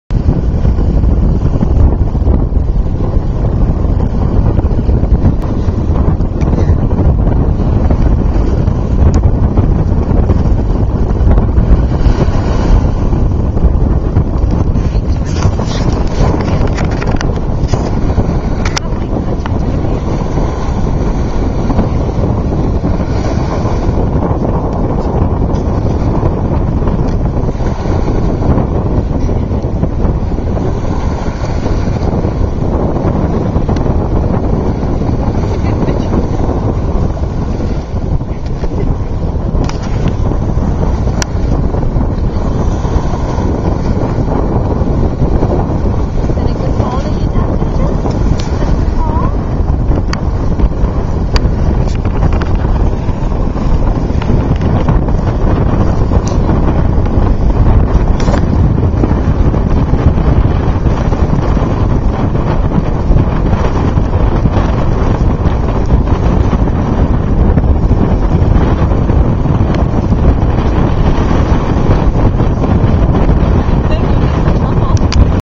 Joey @ Popham Beach
Joey the Cardigan Welsh Corgi playing for the first time on the beach!